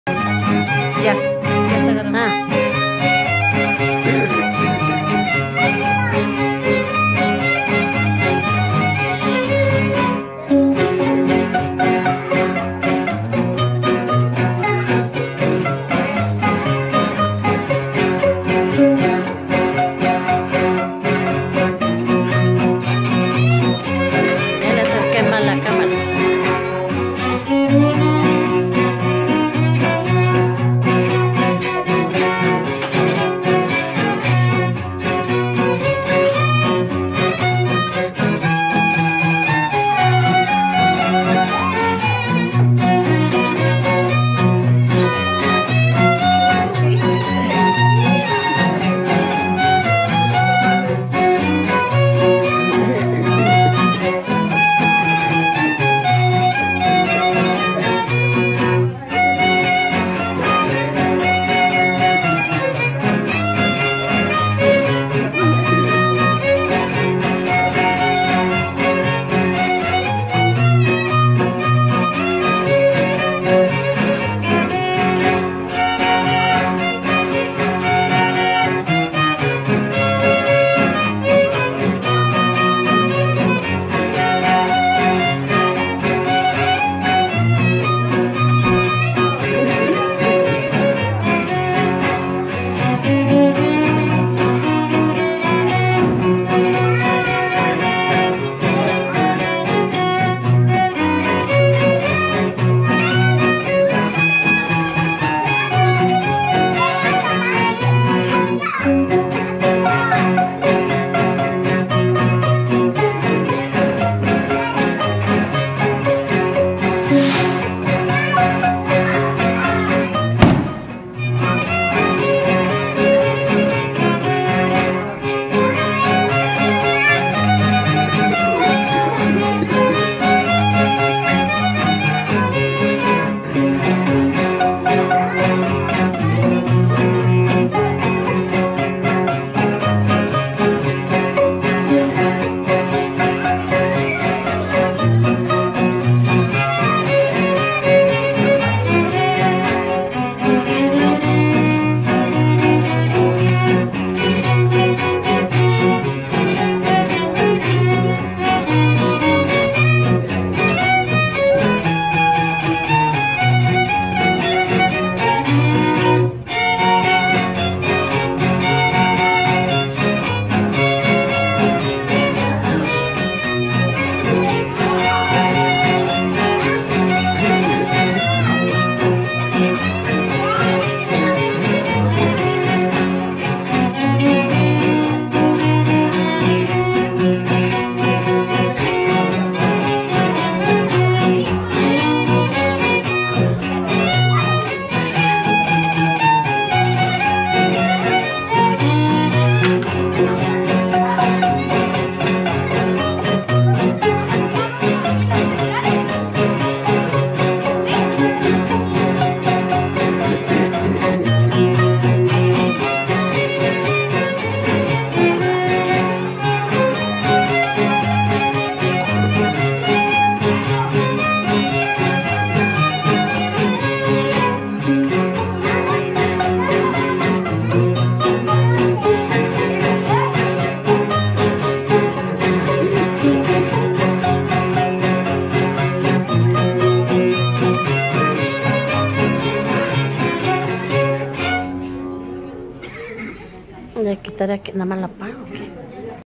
Los hermanos Romero
En este video encontraras uno de mejores temas y melodias de la musica mexicana  tambien conoseras sus tradicione y costumbres. De la sona centro de San luis potosi Mexico